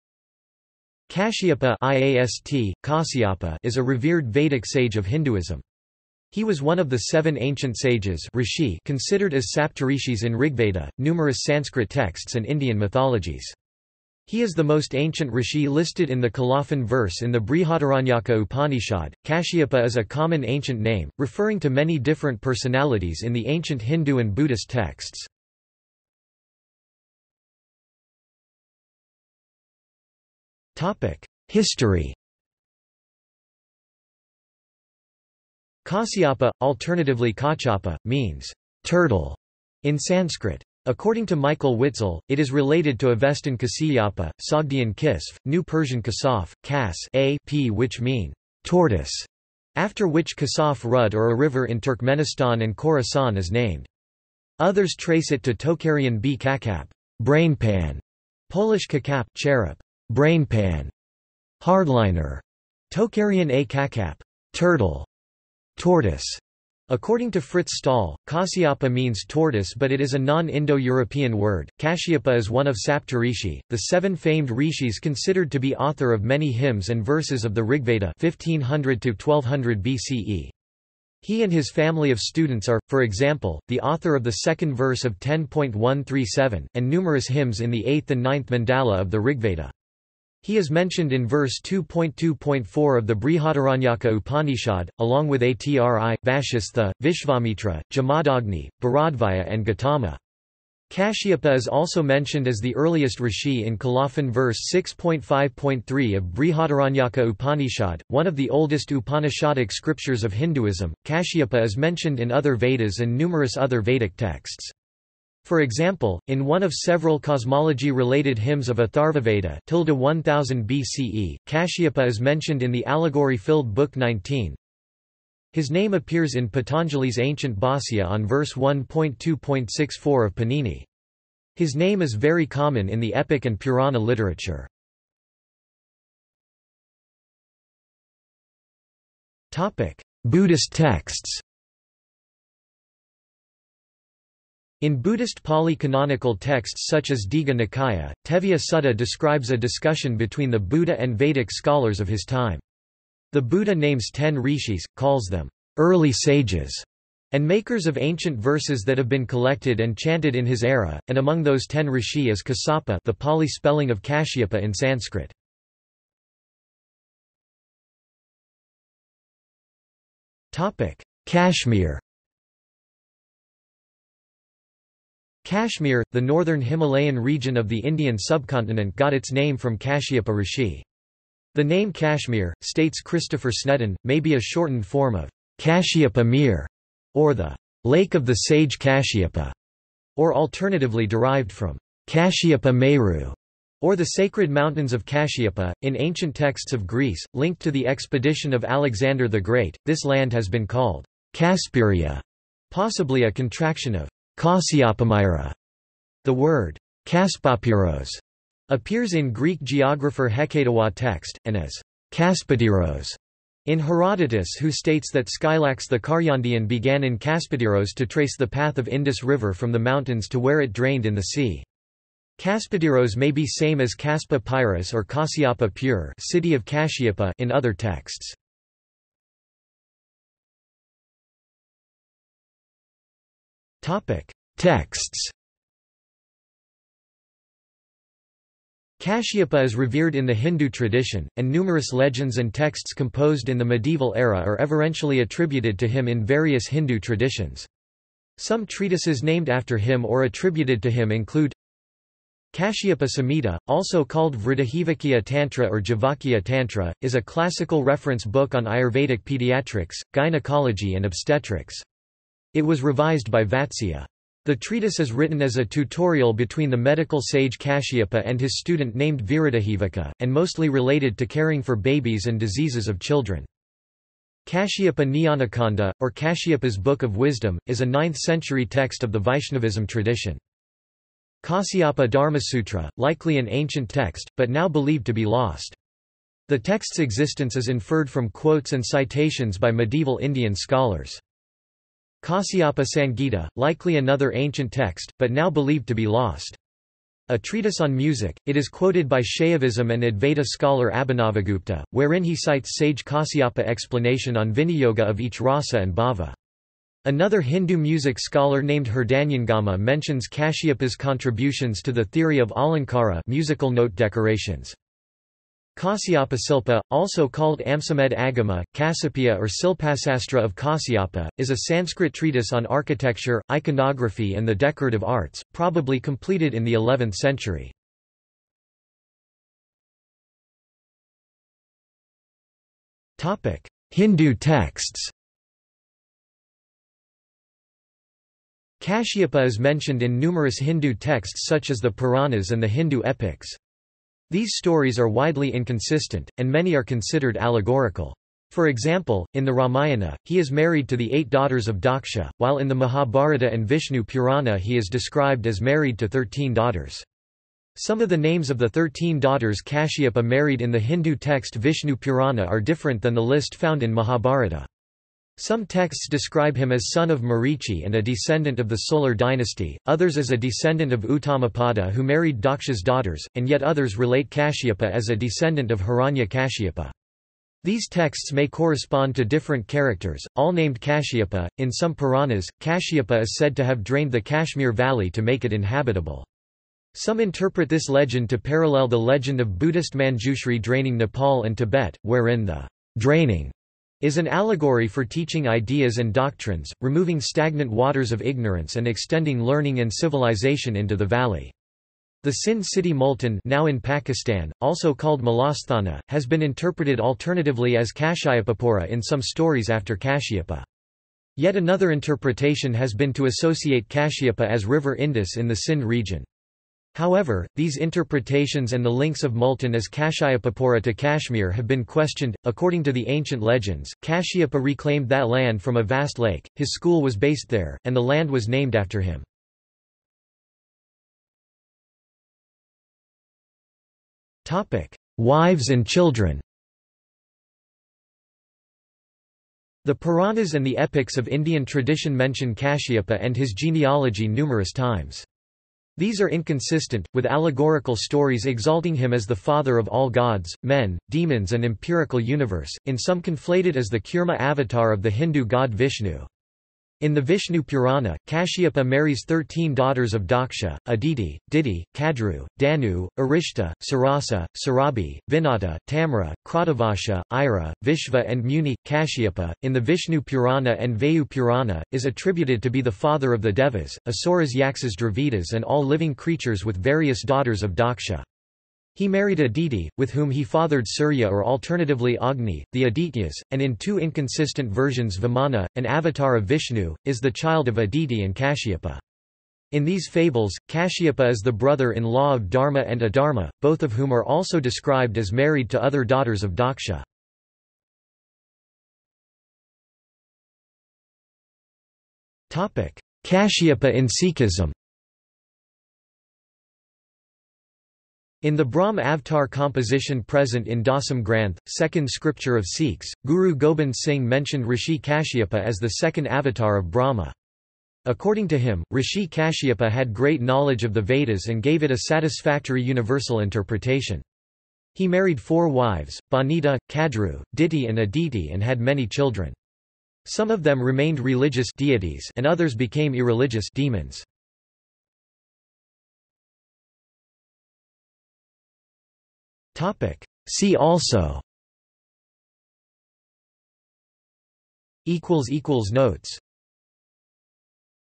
Kashyapa | Wikipedia audio article
This is an audio version of the Wikipedia Article:
Kashyapa

00:00:27 1 History
00:02:16 1.1 Buddhist texts
00:02:47 1.2 Kashmir
00:03:50 2 Texts
00:05:29 3 Hindu texts
00:07:11 3.1 Wives and children
00:08:18 4 Kashyapa in Sikhism
00:08:56 5 See also

- Socrates

SUMMARY
Kashyapa (IAST: Kaśyapa) is a revered Vedic sage of Hinduism. He was one of the seven ancient sages (rishi) considered as Saptarishis in Rigveda, numerous Sanskrit texts and Indian mythologies. He is the most ancient rishi listed in the colophon verse in the Brihadaranyaka Upanishad.Kashyapa is a common ancient name, referring to many different personalities in the ancient Hindu and Buddhist texts.